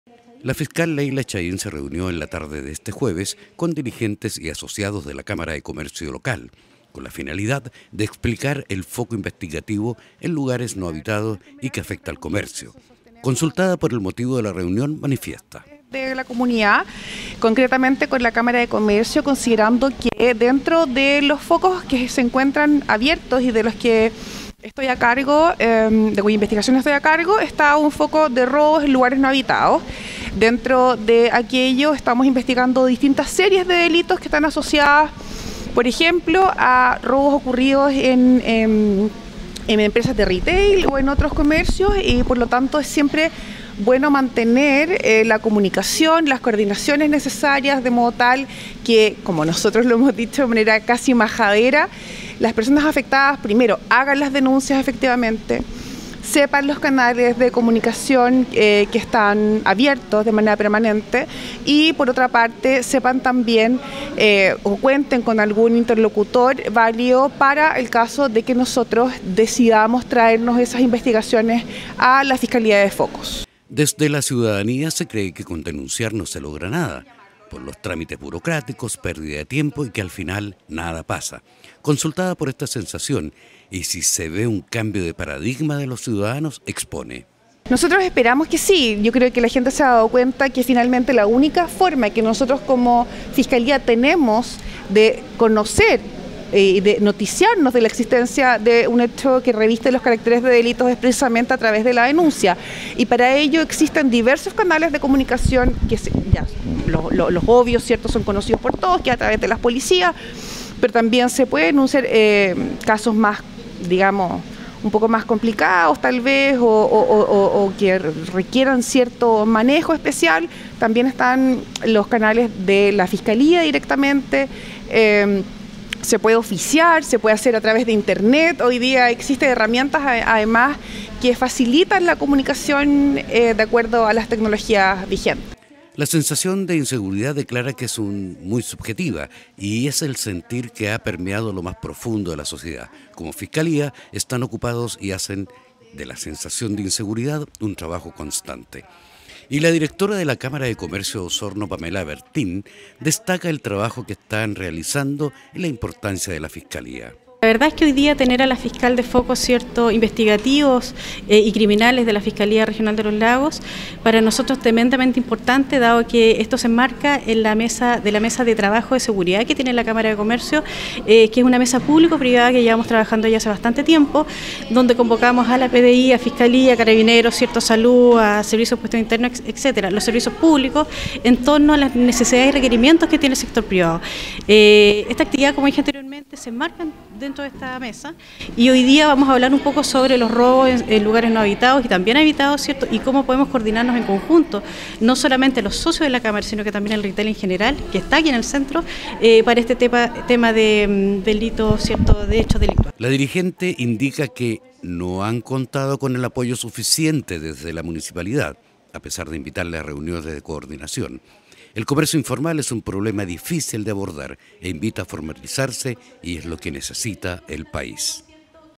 La fiscal Leyla Chahín se reunió con la cámara de comercio de Osorno.
La fiscal Leyla Chahín se reunió en la tarde de este jueves con dirigentes y asociados de la cámara de comercio local. La sensación de inseguridad declaró que es muy subjetiva y que ha permeado a lo más profundo de la sociedad.